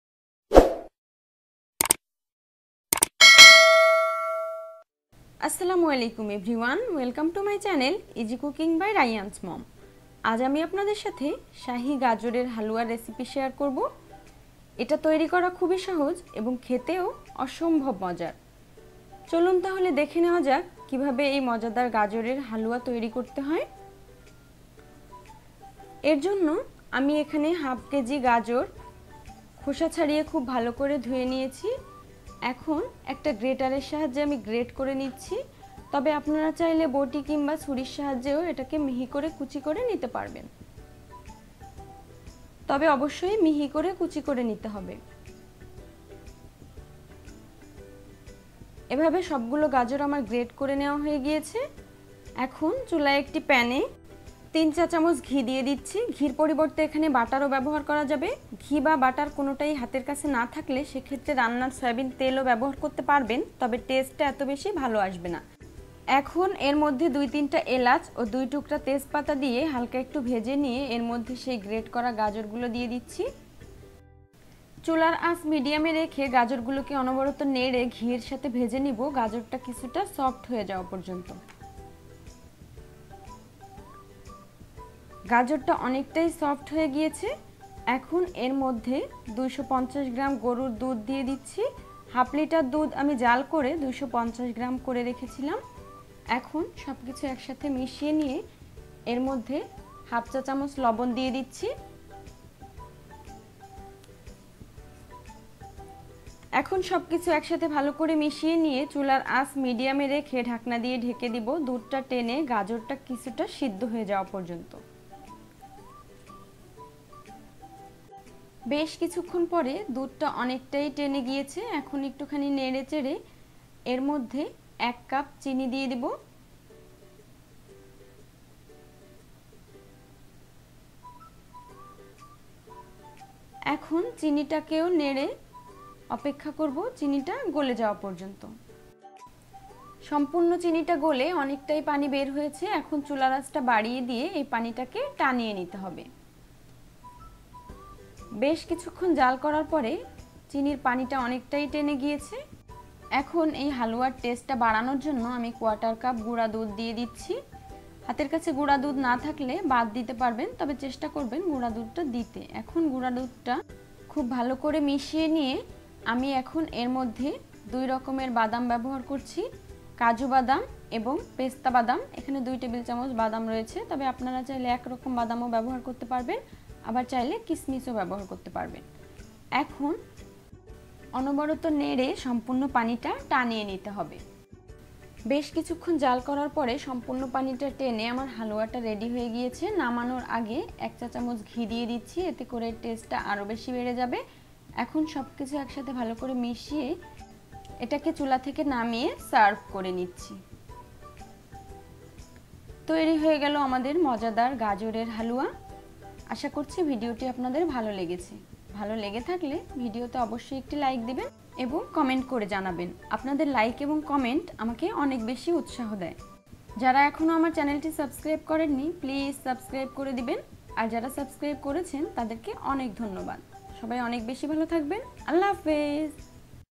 গাজরের হালুয়া/শাহী গাজরের হালুয়া/ Sahi Gajorer Halua/Carrot Halwa/Gajor Ka Halwa/Halwa Recipe //
Ingredients :

*carrot 1/2 kg
*ghee 3 tsp
*hot spicy 2-3 pcs
*powder milk 1/4 cup
*kuju&pesta nut 2 tbs